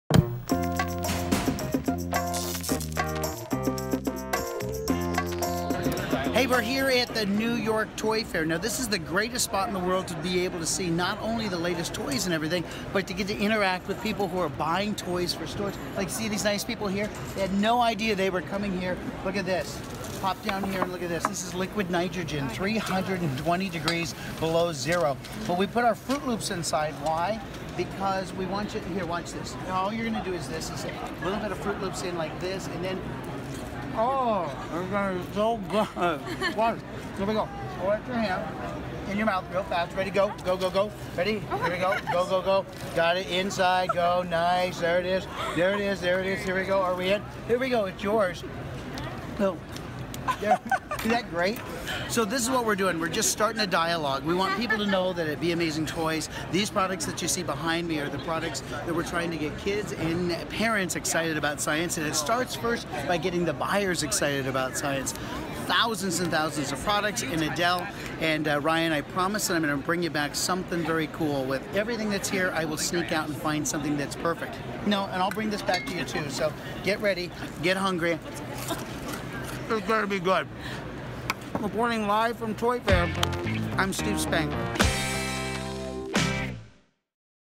Cool Science Stuff at the New York Toy Fair
They call themselves “play professionals” and thousands flock each year to New York to attend the International Toy Fair. Imagine over 400,000 square feet filled with the latest and greatest toys and youth entertainment products. It only makes sense that our own “play professional” Steve Spangler is at Toy Fair finding the coolest toys that make science fun.